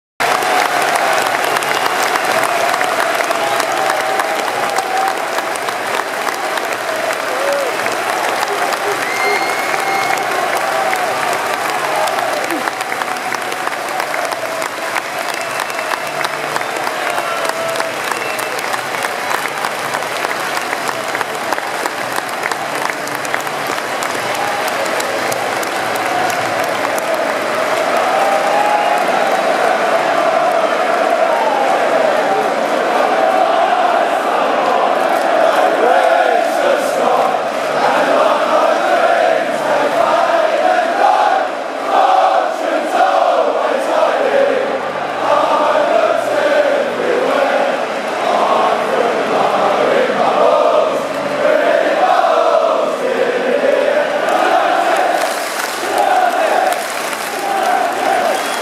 newcastle vs west ham teams coming on to pitch 24/8/2013
newcastle vs west ham team at st james`s park